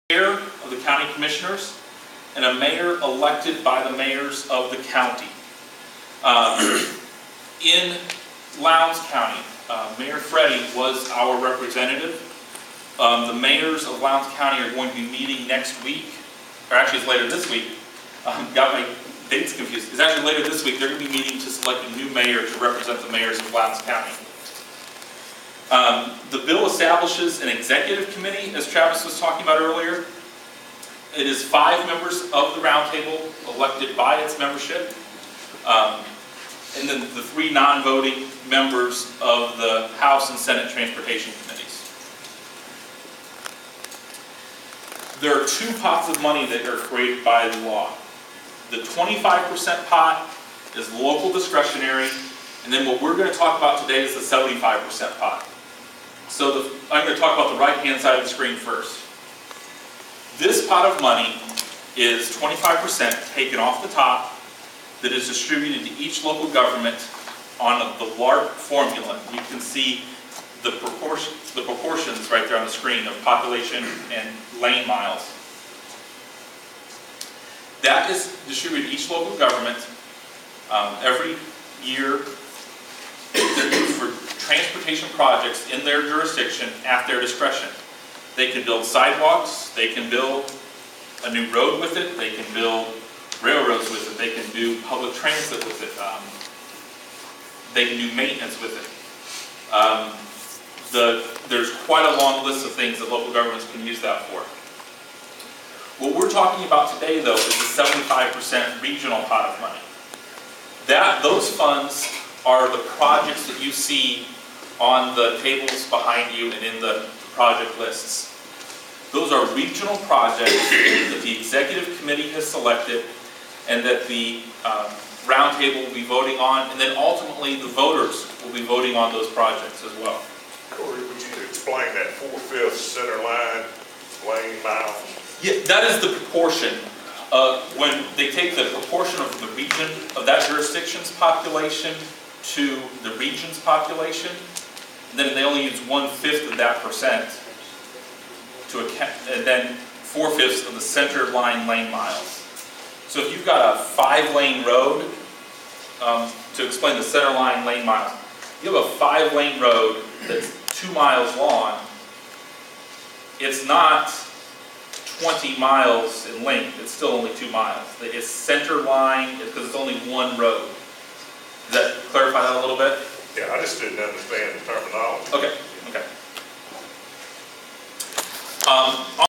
Who is on the Roundtable? --Corey Hull @ VLCIA 2011-09-19
T-SPLOST Public Meeting, Southern Georgia Regional Commission (SGRC)
Corey Hull, Travis Harper,